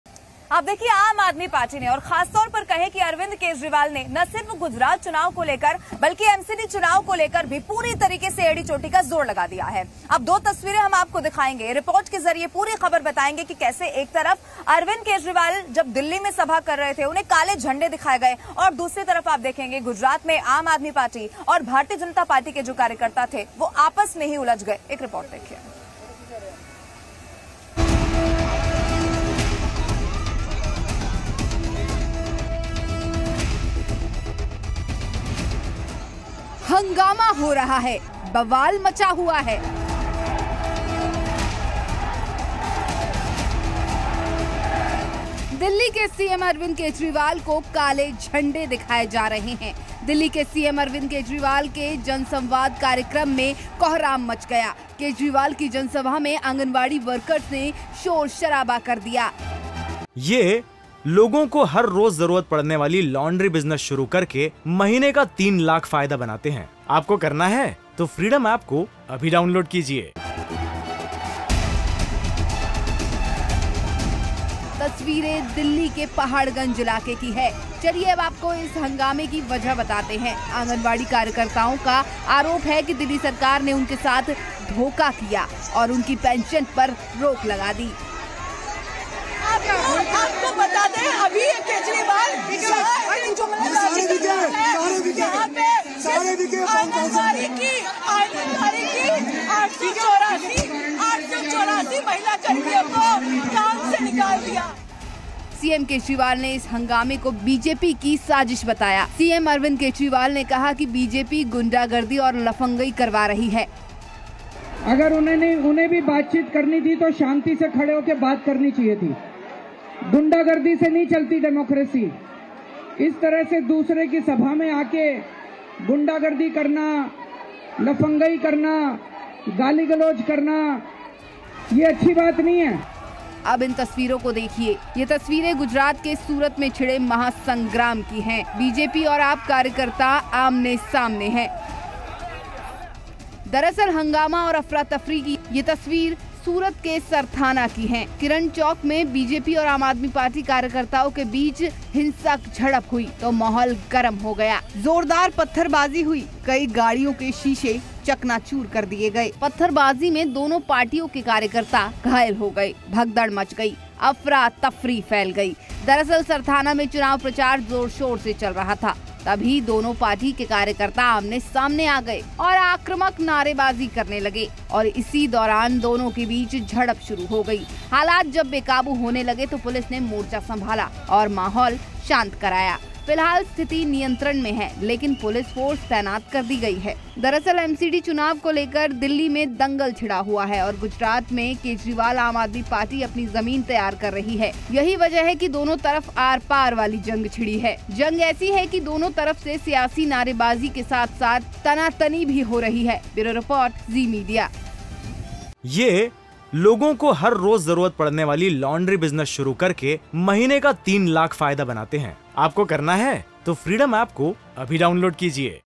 Mera Rajya Mera Desh: Kejriwal के जनसंवाद कार्यक्रम में क्यों मचा हंगामा ? | MCD Election |AAP Rally
एमसीडी इलेक्शन को लेकर दिल्ली के पहाड़गंज में एक पब्लिक रैली को संबोधित करने पहुंचे मुख्यमंत्री अरविंद केजरीवाल को भारी विरोध का सामना करना पड़ा. हंगामा आंगनबाड़ी कार्यकर्ता कर रही थीं. दरअसल, आगंनबाड़ी कार्यकर्ताओं का आरोप है कि, केजरीवाल सरकार ने उनके साथ धोखा किया और उनके पेंशन पर रोक लगा दी. साथ ही Aanganbadi Workers ने आरोप लगाया कि 884 आंगनबाड़ी कार्यकर्ताओं को नौकरी से निकाल दिया गया है.

About Channel